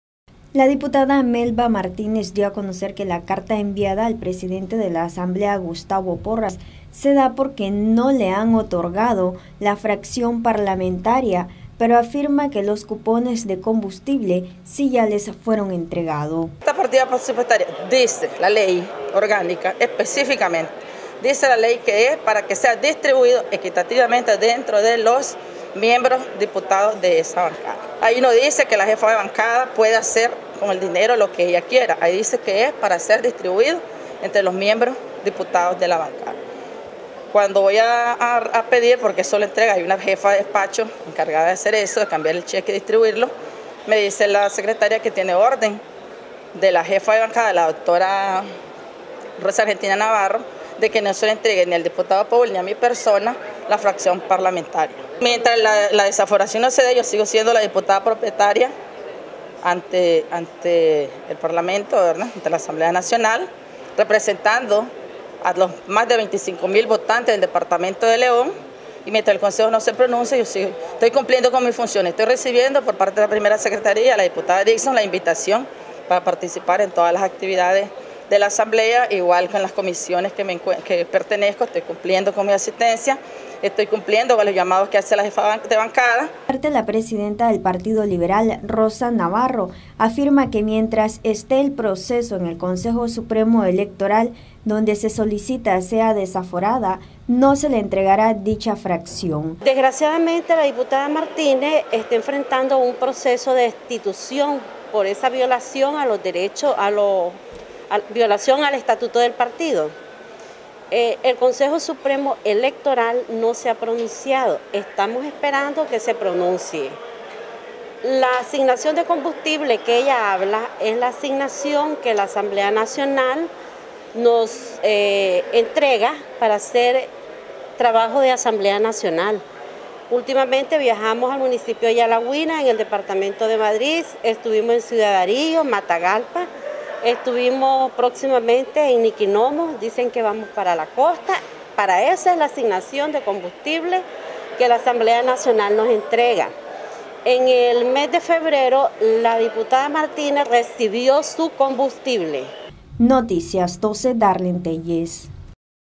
Diputada Liberal demanda les sea devuelta la Fracción Parlamentaria
NosResistimosAlEmbargo Mientras haya una solicitud en el CSE electoral para ser desaforada del parlamento la diputada Melba Martínez, no se le entregará la fracción, dijo la presidenta de esa bancada Rosa Argentina Navarro.